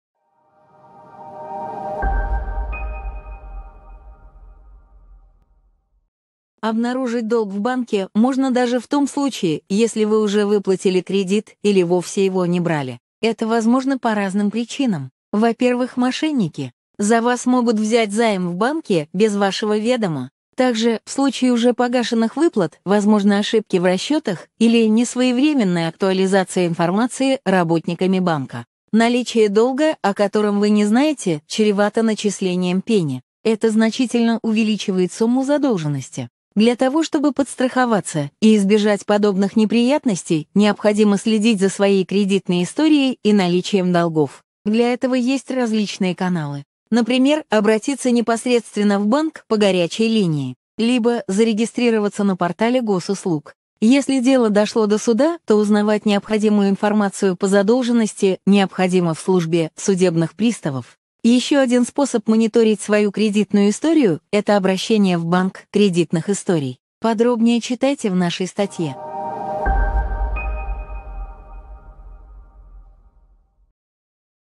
Как узнать есть ли долг в банке
Для того, чтобы обезопасить себя от неожиданных долгов перед банком, даже если вы уже выплатили кредит, или вовсе его не брали, необходимо мониторить информацию о должниках